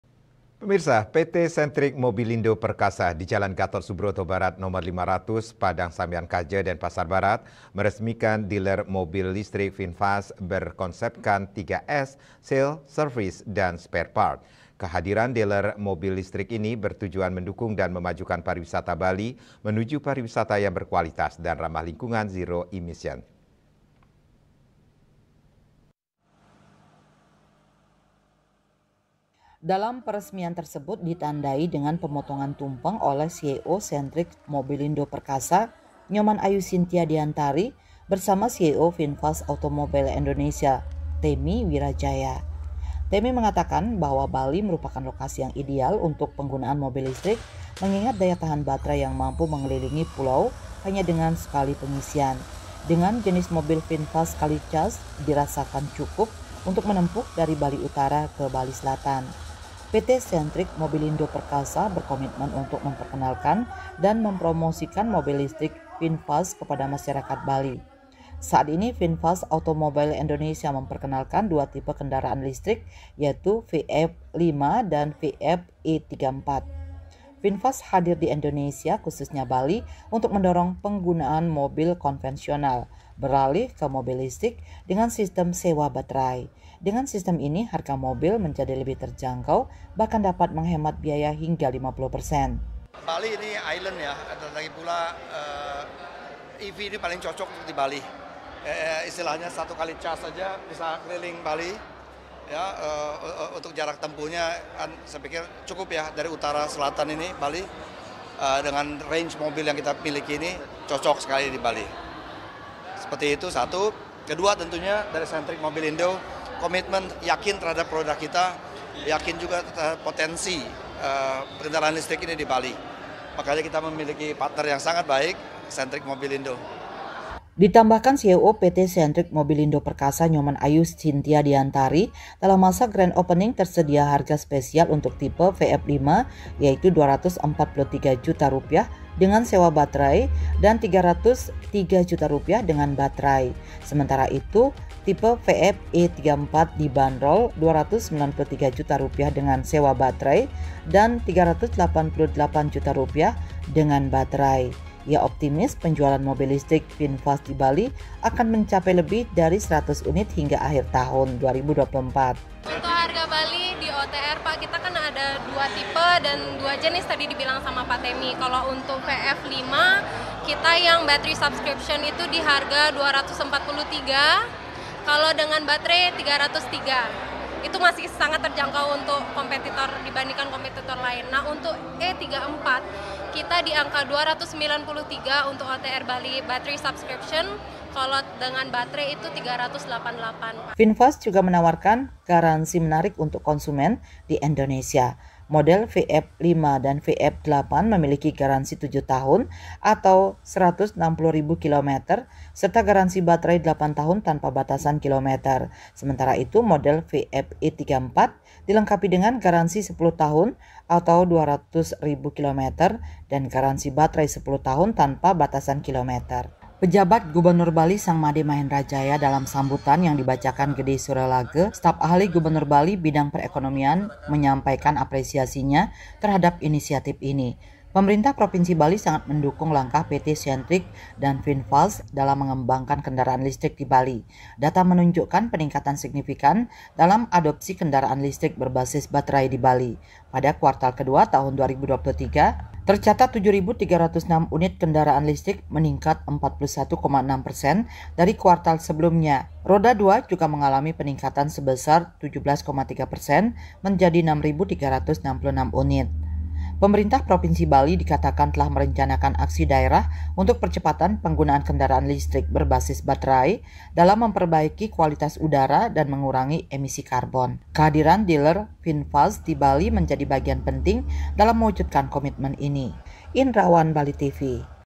PT. SENTRIK MOBILINDO PERKASA RESMIKAN DEALER MOBIL LISTRIK VINFAST
PT SENTRIK MOBILINDO PERKASA DI JALAN GATOT SUBROTO BARAT NOMOR 500 PADANG SAMBIAN KAJA, DENPASAR BARAT, MERESMIKAN DEALER MOBIL LISTRIK VINFAST BERKONSEPKAN 3-S, SALE, SERVICE, DAN SPAREPART. KEHADIRAN DEALER MOBIL LISTRIK INI BERTUJUAN MENDUKUNG DAN MEMAJUKAN PARIWISATA BALI, MENUJU PARIWISATA YANG BERKUALITAS DAN RAMAH LINGKUNGAN ZERO EMISION.